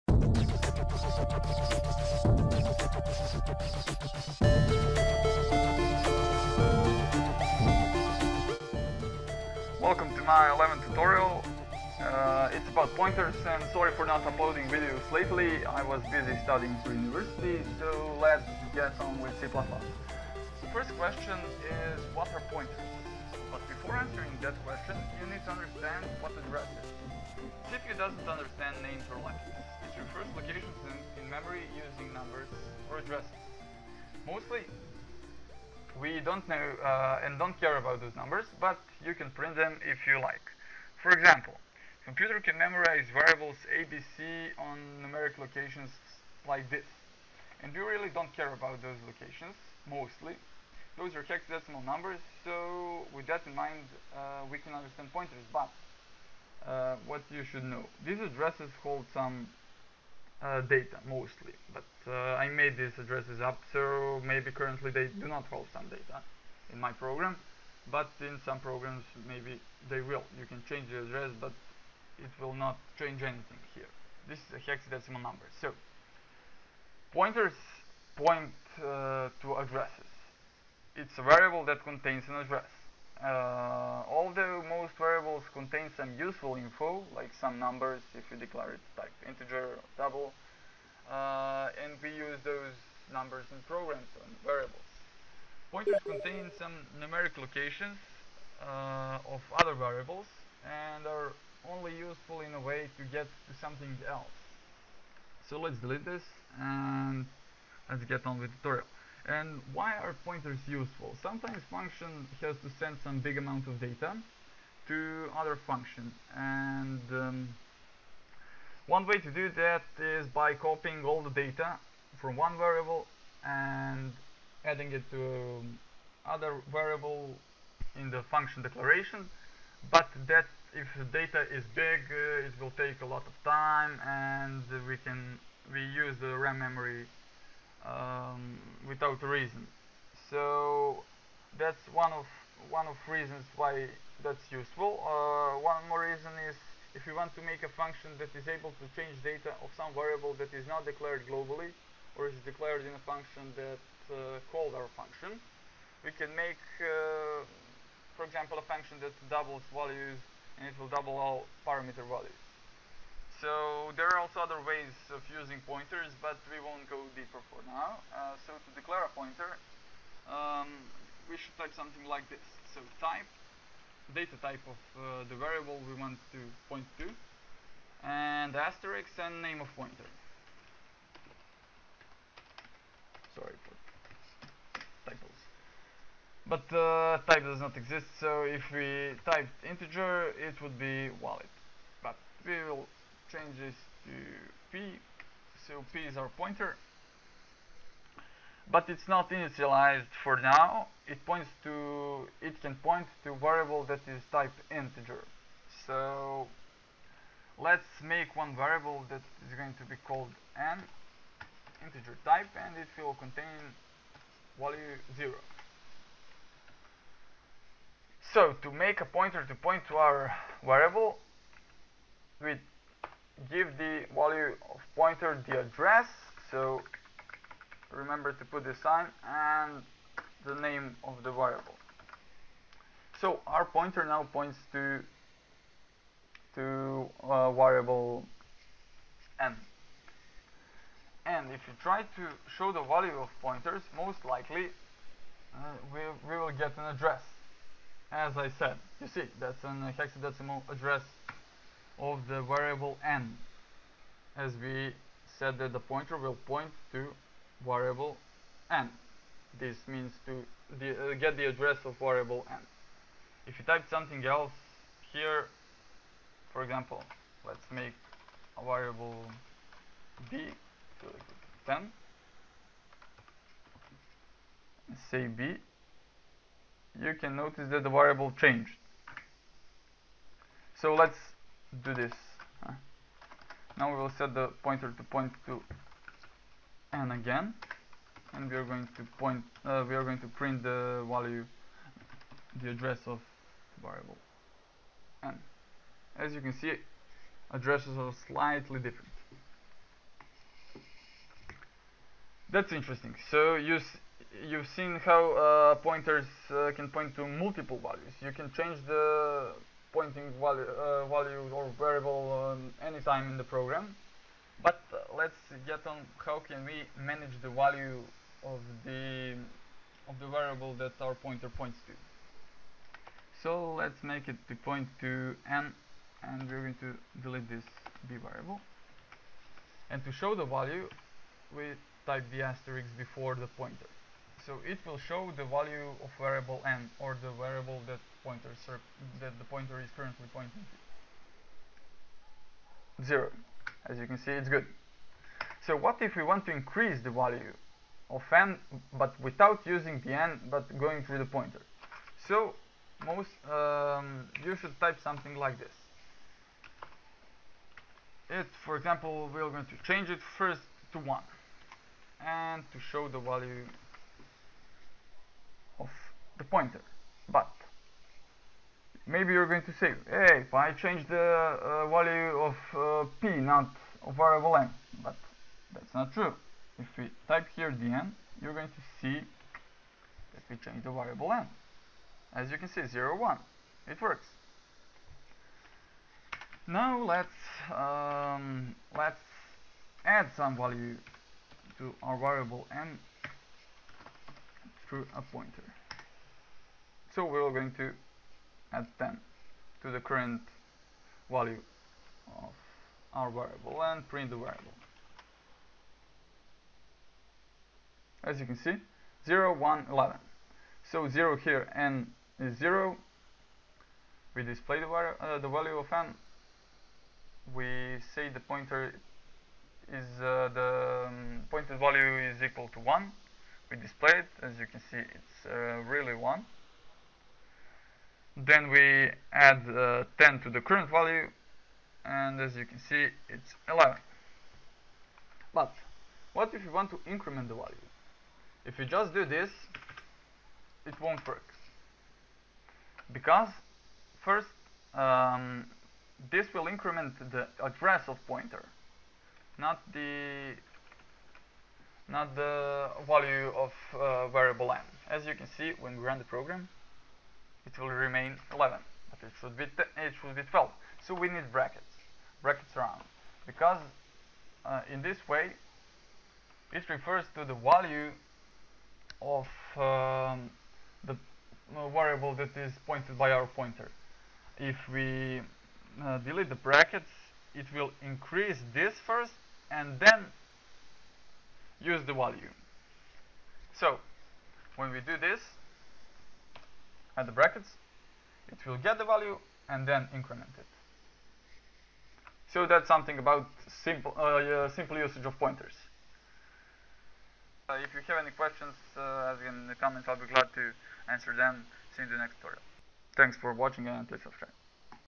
C++ Tutorial 11 - Pointers
Welcome to my simple tutorial on C++. In this tutorials you will learn simple thing for using pointers and manipulating with them.

If you have any questions I'll be glad to answer, please leave a comment on the video.

Great C++ books that I recommend for beginners:
C++ Without Fear: A Beginner's Guide That Makes You Feel Smart (3rd Edition)